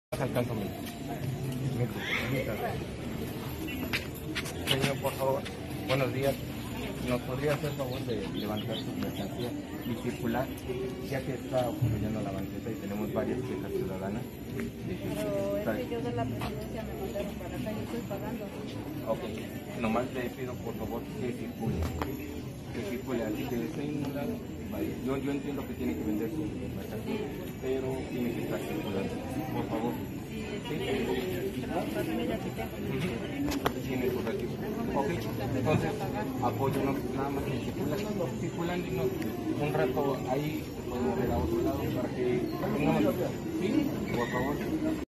Con granaderos el gobierno municipal de San Pedro Cholula operativo sorpresa para liberar banquetas
Con granaderos, realiza el gobierno municipal de San Pedro Cholula operativo sorpresa para liberar banquetas y calles de vendedores ambulantes, en los alrededores del mercado municipal Cosme del Razo.